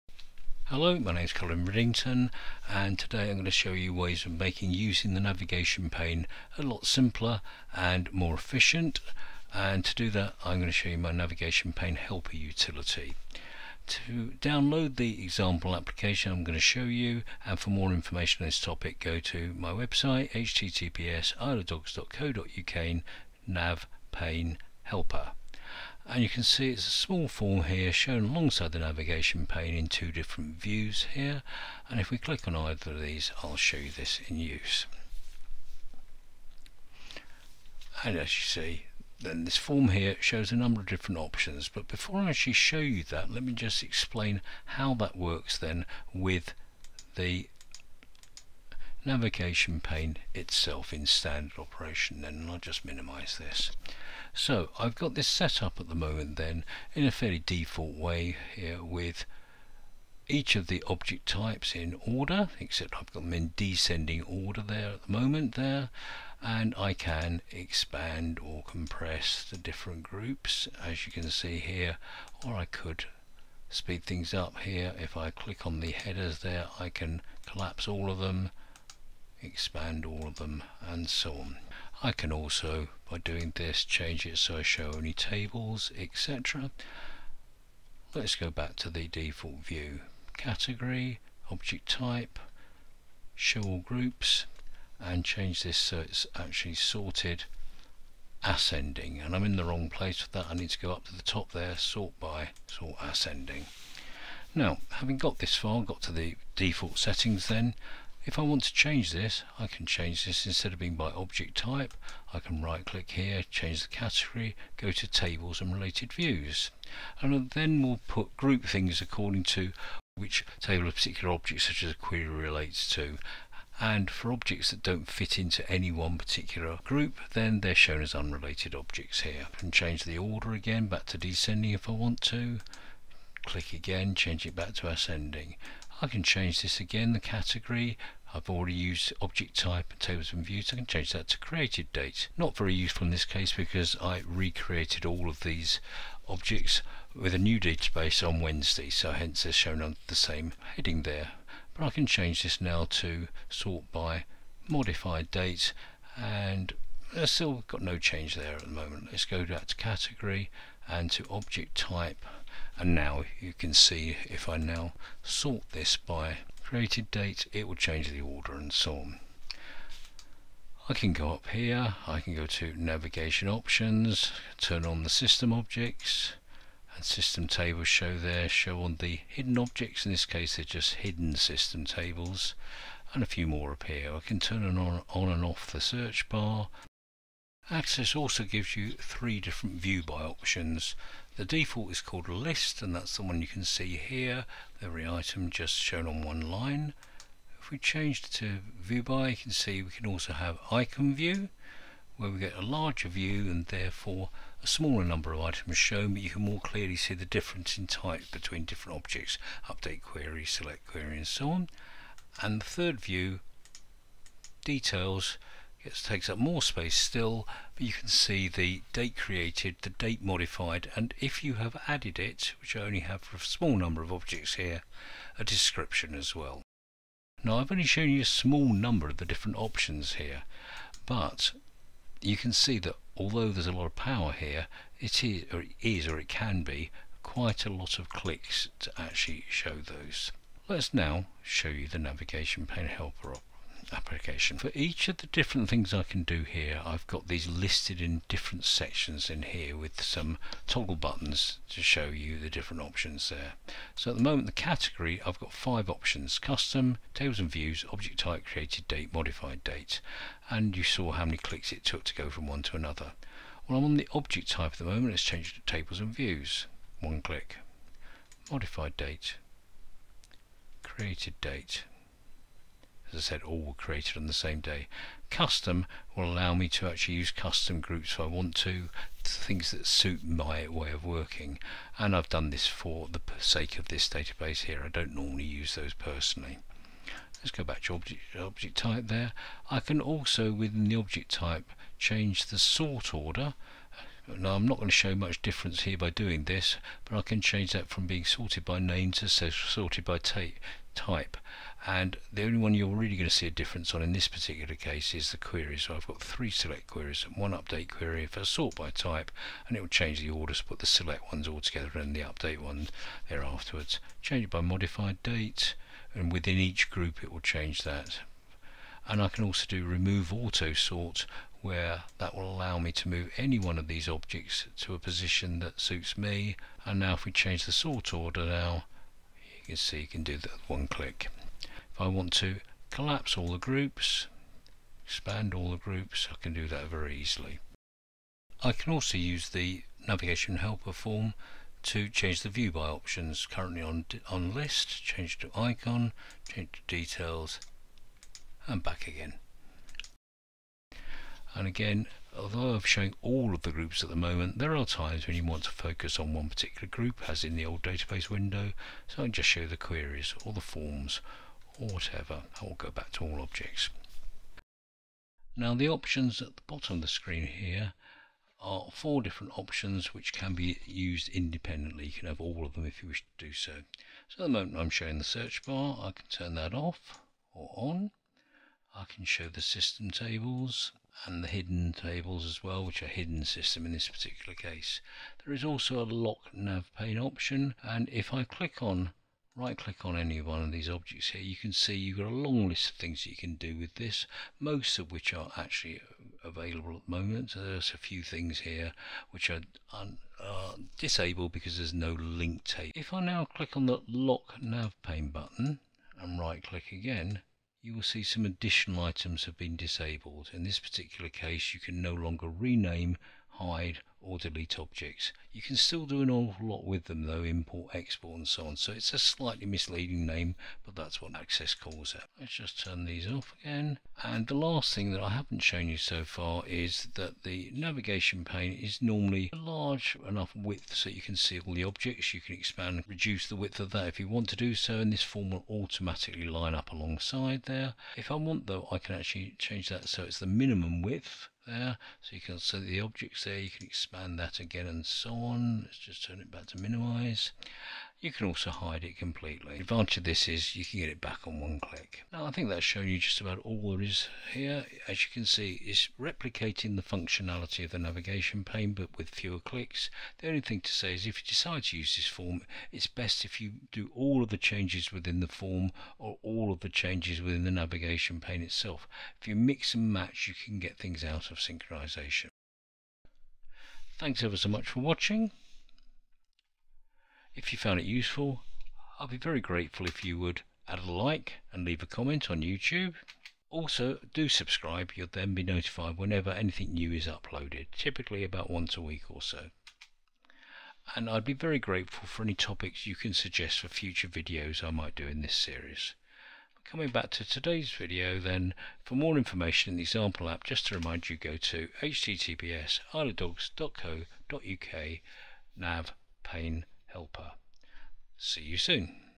Navigation Pane Helper
The navigation pane helper utility was designed to replicate the huge number of options available from the navigation pane but to do so more efficiently with as few clicks as possible

For more information and to download the free example app, go

TIMES:
0:00 Intro and download link
0:38 Example app
0:54 Exploring various navigation pane options
1:06 Expand/collapse groups; show selected group only
1:35 Change sort order - ascending/descending
1:50 Change category - object type/tables & related views/modified date etc
2:55 Navigations pane options - Show/hide system objects/hidden objects/search bar
3:07 View by options - list/icon/details
3:47 Many different display options but can take many clicks in some cases
4:02 Nav Pane Helper form - replicates nav pane display options but with one click
4:20 Change category options
4:49 Change Sort By options
5:26 Change Sort Order
5:31 Expand/Collapse Groups
5:38 Change View By options
5:50 View Object Options
6:05 Nav Pane Options - including 'Lock Navigation Pane'
7:04 Change Nav Pane width - maximize/minimize/hide
7:40 Best to change settings entirely in helper form or nav pane - avoid 'mix & match'
7:53 Thanks for watching ...